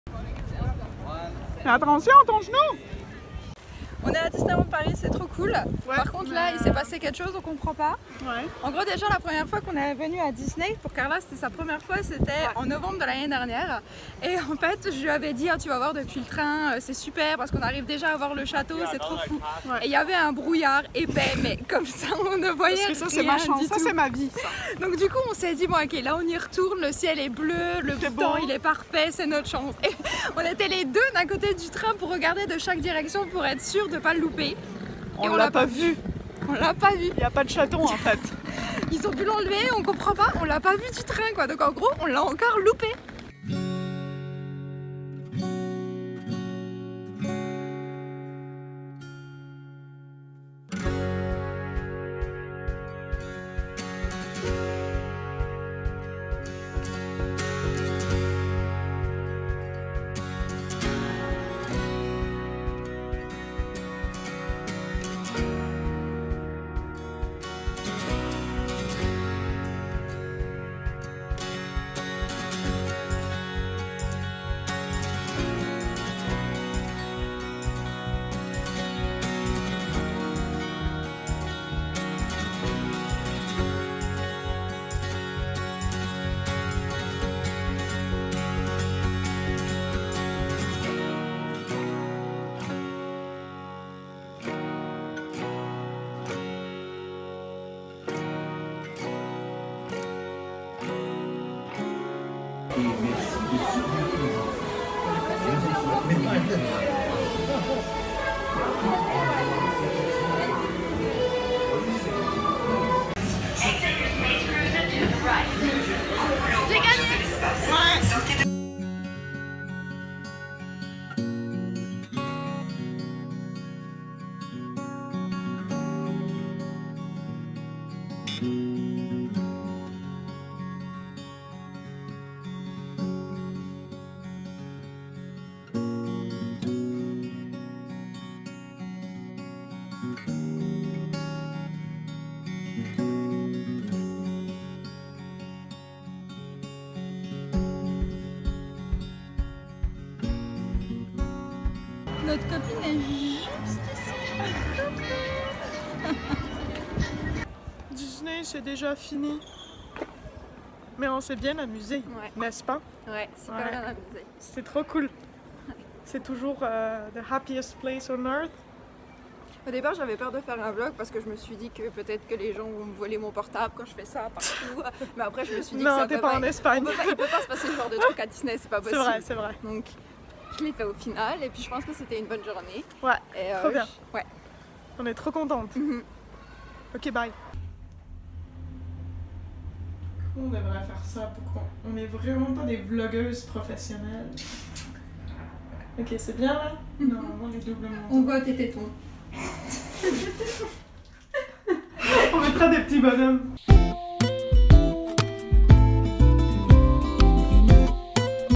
VLOG À DISNEY !
SALUT ! On est de retour sur Girl Talk avec un Vlog à Disney ! Merci d'être encore là, on est vraiment pas bonnes HAHA. On vous promet qu'on va continuer avec cette chaîne. On a d'ailleurs plein de nouvelles idées et c'est un peu différent de ce qu'on fait d'habitude, mais on pense que ça va vous plaire !

► CARLA carlarosa7
► LOUISIANE louisiane13

Pour info : Est-ce que quelqu'un a déjà vu le château de Disney depuis le train ? Ou c'est juste Louisiane qui est folle ?